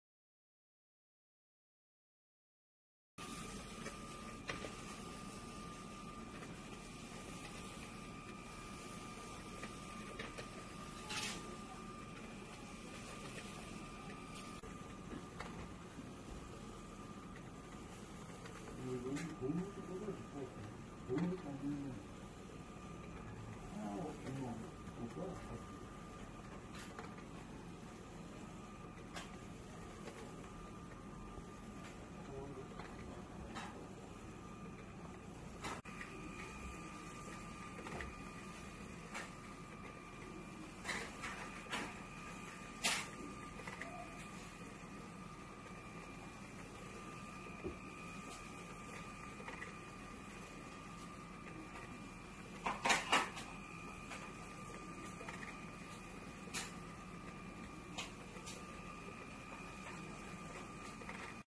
LEGO #45678 Spike Prime Python Drawing machine
Draw a picture using rotary motion as a reciprocal motion.